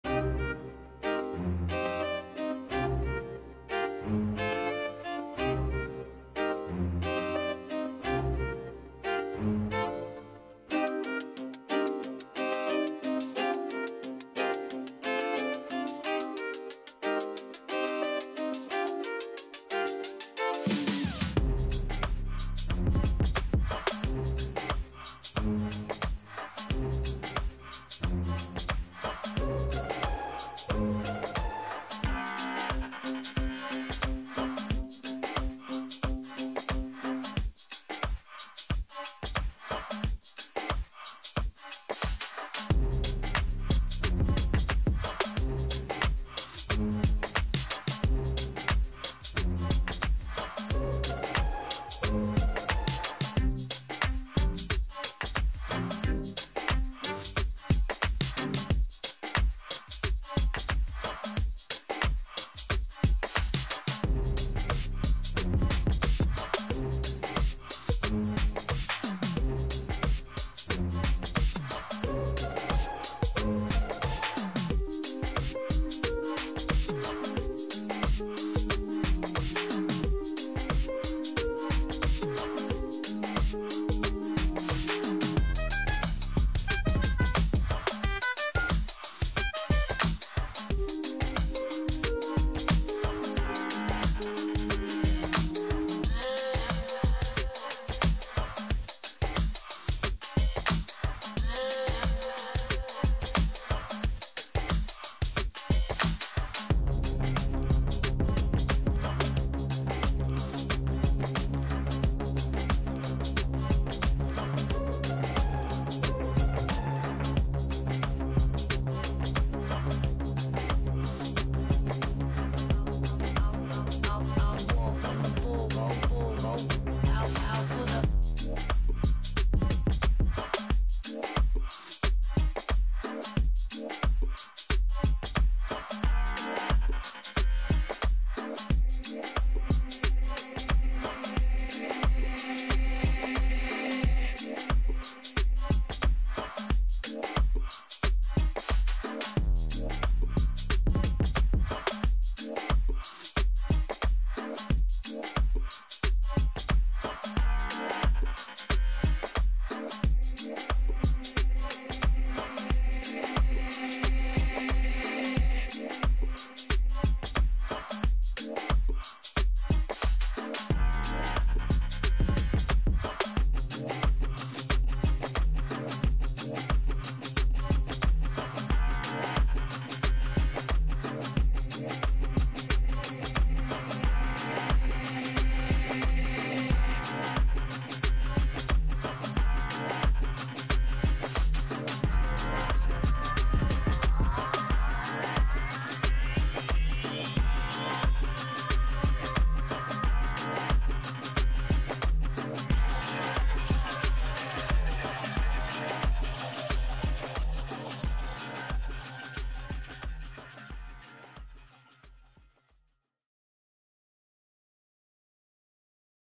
Classic Takes Over
Pictures from Sonsbeek Park in Arnhem (Netherlands). 90 BPM, so it is suitable for supporting your sports